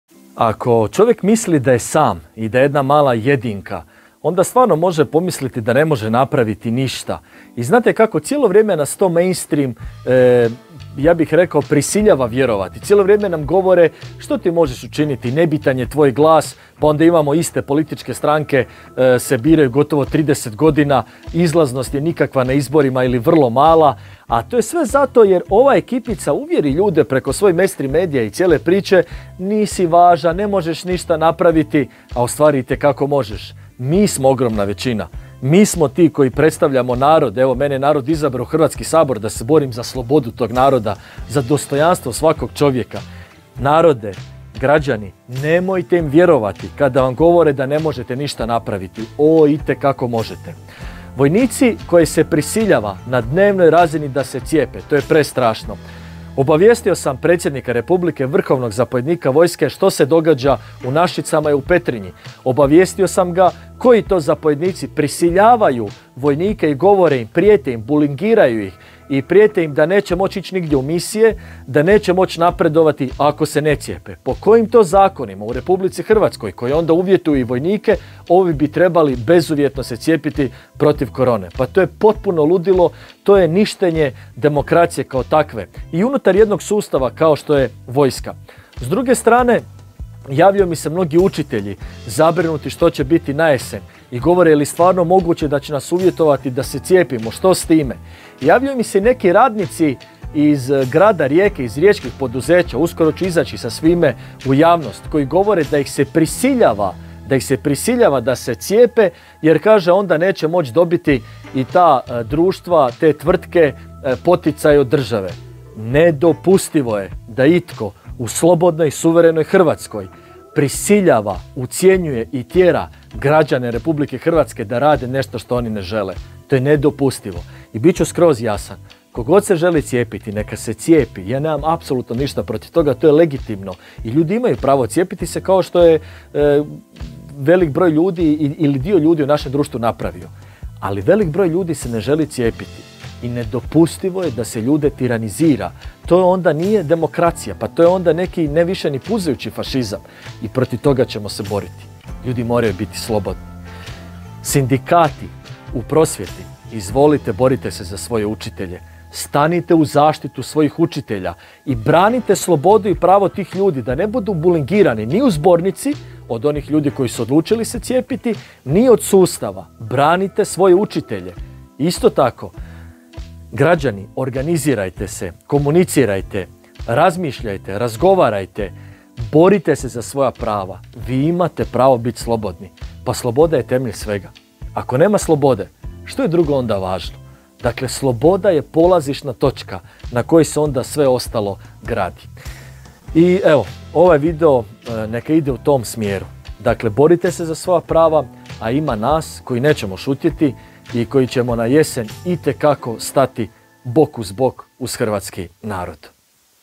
Građani su slobodni i slobodu nam NITKO neće oduzeti‼️
Nemojte misliti da ste sami‼️ Niste sami, mnogo nas je. Većina nas je. I obranit ćemo slobodu.

Poslao sam Predsjedniku Republike informacije o tlačiteljima u vojsci koji ucjenjuju vojnike da se moraju cijepiti protiv COVID-a ili neće moći ići u misije i napredovati. Prijete i učiteljima kao i nekim tvrtkama u Rijeci da neće dobiti poticaje od države ako se ne cijepe pa će radnicima dati otkaz.

Budete li nastavili tlačiti narod ucjenama - narod vam neće na to šutjeti.